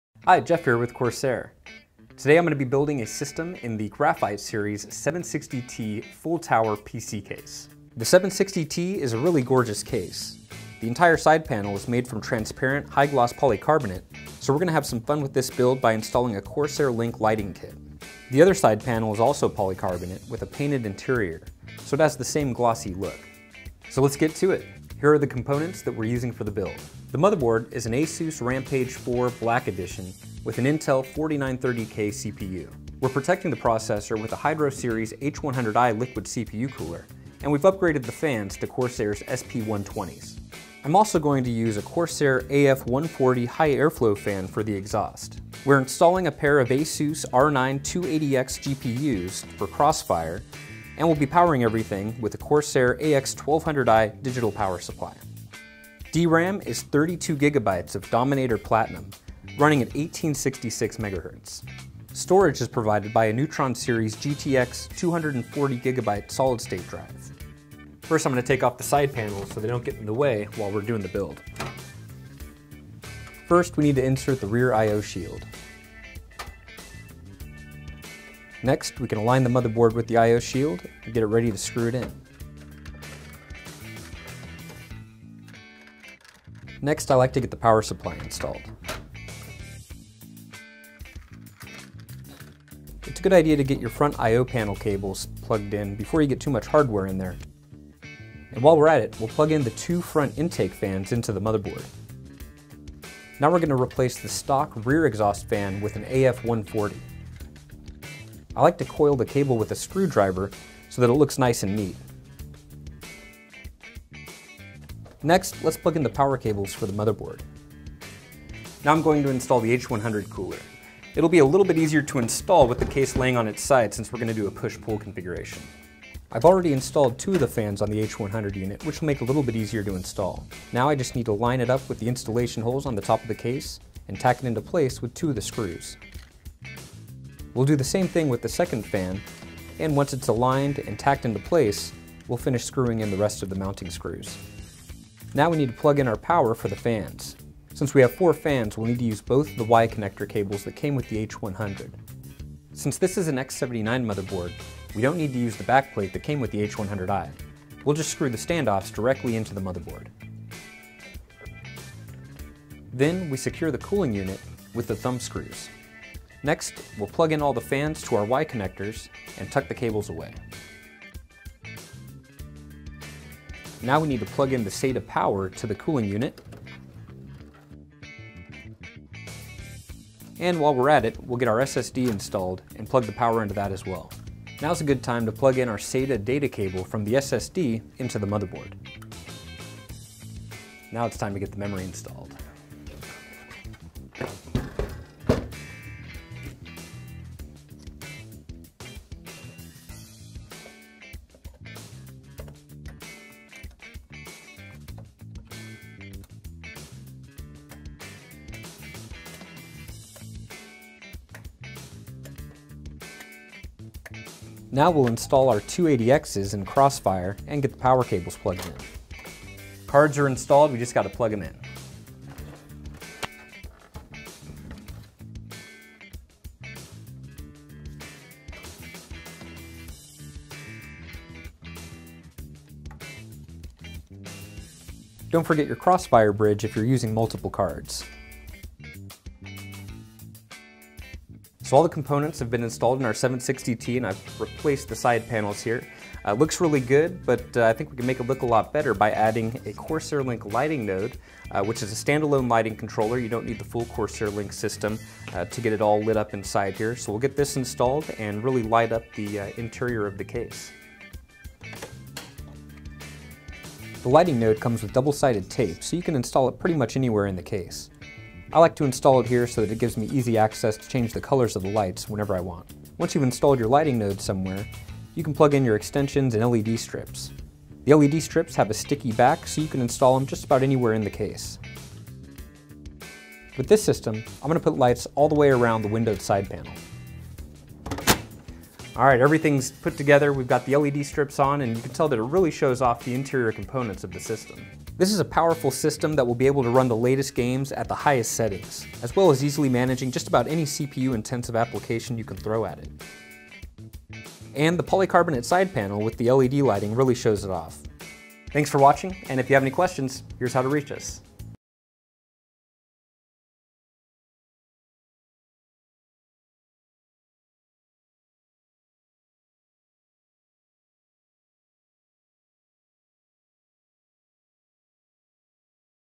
How To Build a PC in the CORSAIR Graphite Series 760T
Jeff Checchi takes the Graphite Series 760T for a spin by building a system with an ASUS Rampage IV Black Edition motherboard, dual ASUS Radeon R9 280X GPUs in Crossfire, and lots of CORSAIR components including an AX1200i digital ATX power supply, Dominator Platinum DRAM, a Hydro Series H100i liquid CPU cooler, and a CORSAIR Link lighting kit to finish it off.